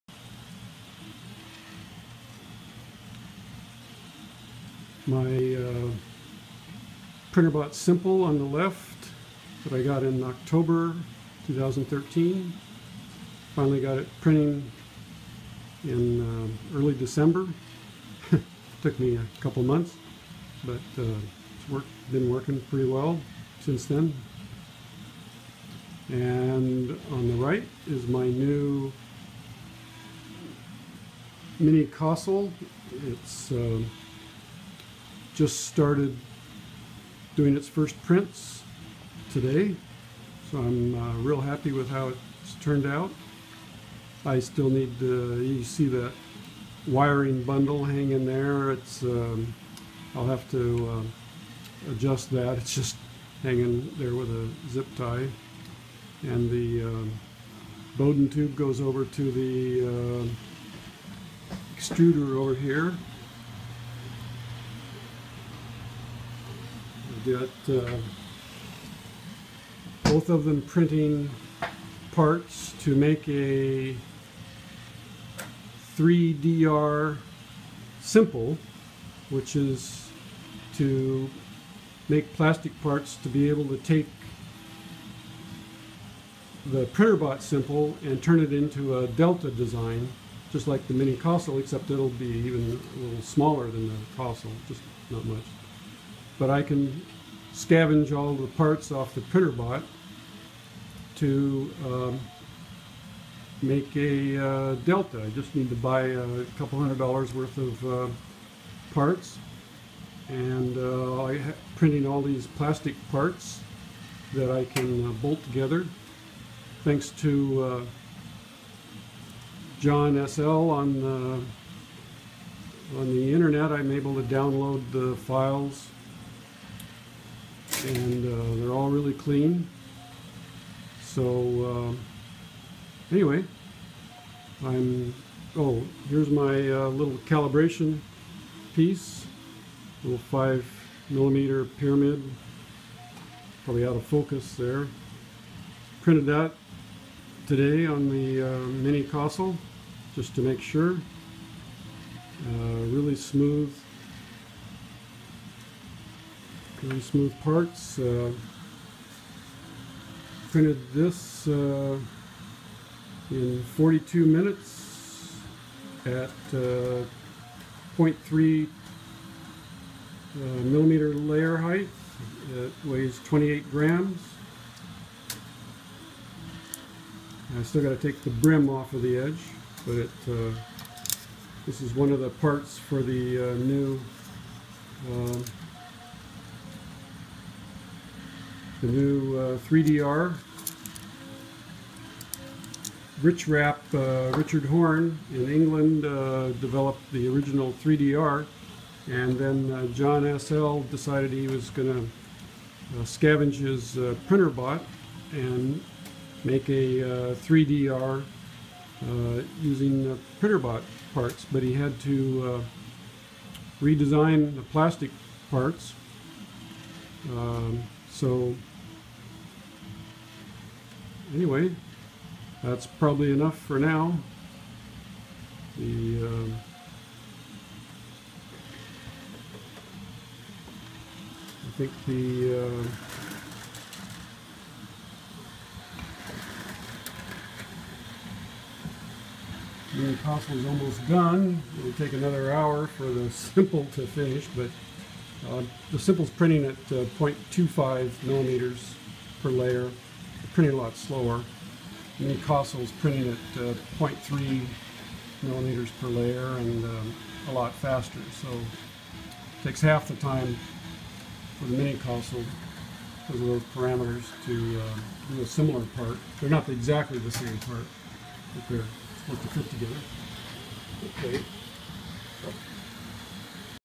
3d printing First Prints Mini Kossel
Printing parts with new Mini Kossel for converting Printrbot Simple to a 3DR Delta designed by JohnSL.  Simple is printing a red version of part that MK is printing blue version.  Decided to print all the future parts on the MK because it is faster and all the same parts will have the same dimensions.